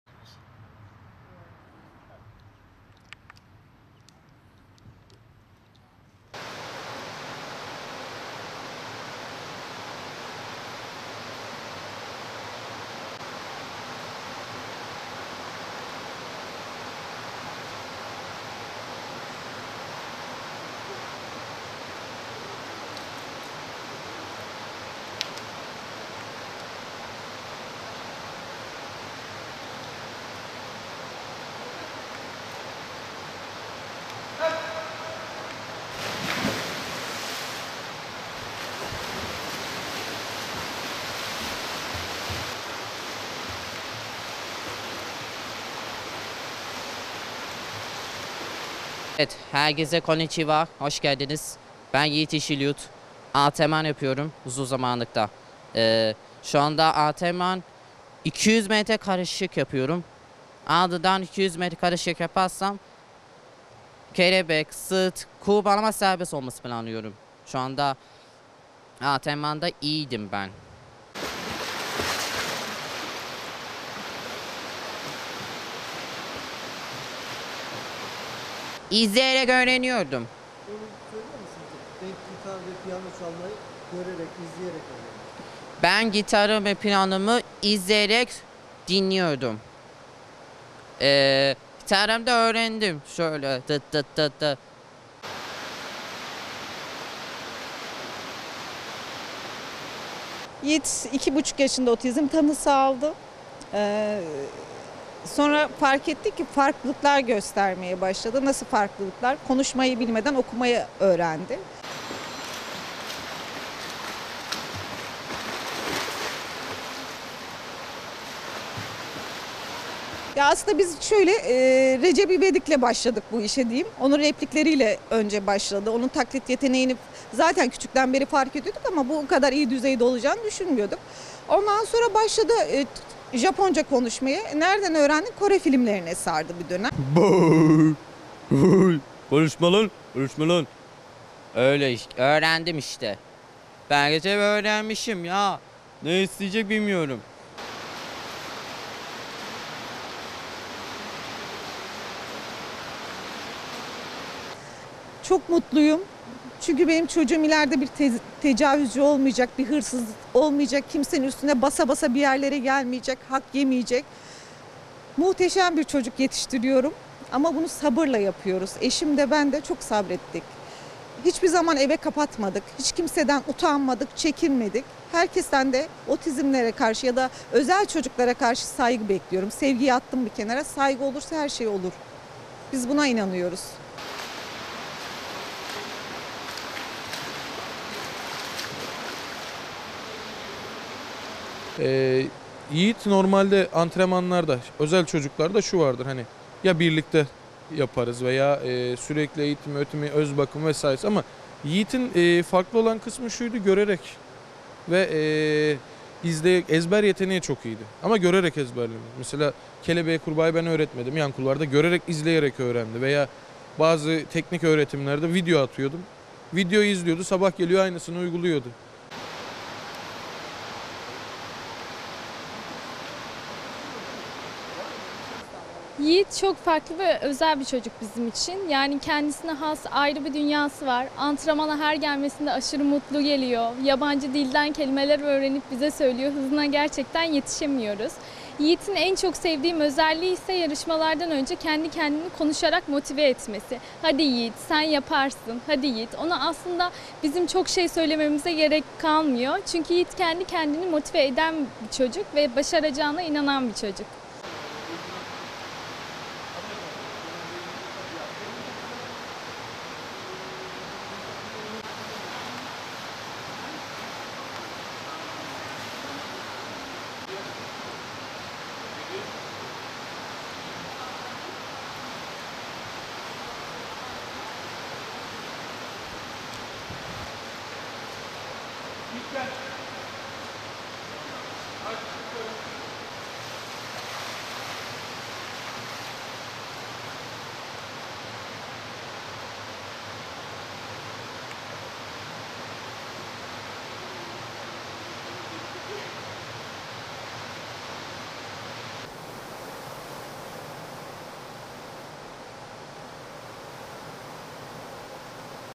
Otizmin Engelini İzleyerek Öğrendiği Yüzmeyle Aşıyor
Trabzon'da otizmli Yiğit Yeşilyurt’un (15) hayatı, 2 yıl önce havuzda izleyerek öğrendiği yüzme sporuyla değişti. Çalıştığı kulüp bünyesinde çıktığı farklı müsabakalarda altın madalyalara uzanıp, Türkiye Şampiyonası’na hazırlanan Yeşilyurt, “Her şeyi izleyerek öğrendim, hedefim şampiyon olmak” dedi.
Aleyna KESKİN- Selçuk BAŞAR/TRABZON, (DHA)-